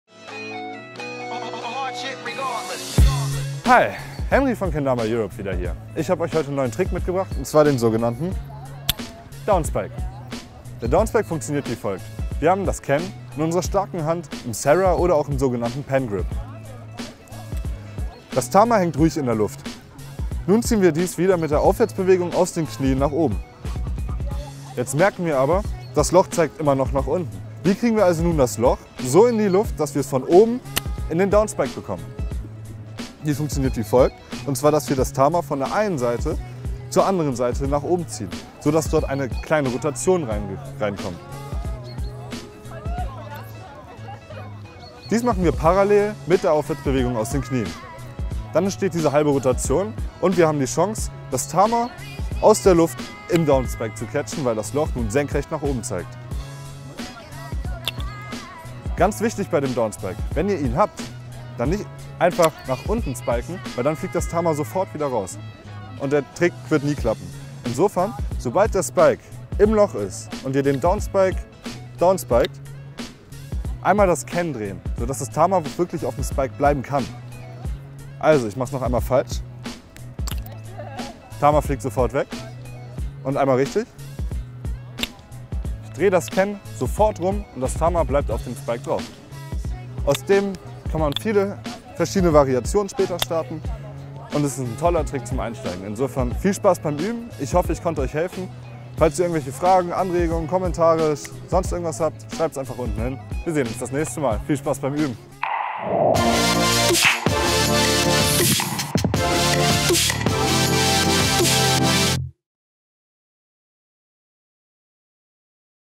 Kendama Downspike - Kendama Europe Tutorial
In unseren vorrigen Tutorials hast Du gelernt wie man normal Spiked und die Kugel auf der Spitze fängt. Heute wollen wir Dir den Downspike zeigen. Gerade wenn man im Sara Grip spiket, ist der Downspike ein super Einstieg für eine neue Combo. Henri gibt Euch die nötigen Tips. Viel Spaß!

Want to learn the Downspike? Henri from Kendama Europe will gives you some great tips and tricks.

Experience the world of Kendama Europe and stay in the loop with all actual Kendama Events, Competitions and news: